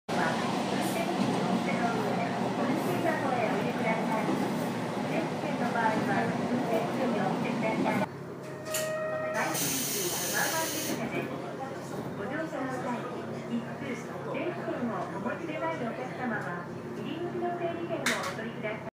ワンマン０４
は運転士にお見せのうえ、運賃箱へお入れください
定期券の場合は運転士にお見せください

行きワンマン電車です
ご乗車の際、切符、定期券をお持ちでないお客様は入り口の整理券をお取りください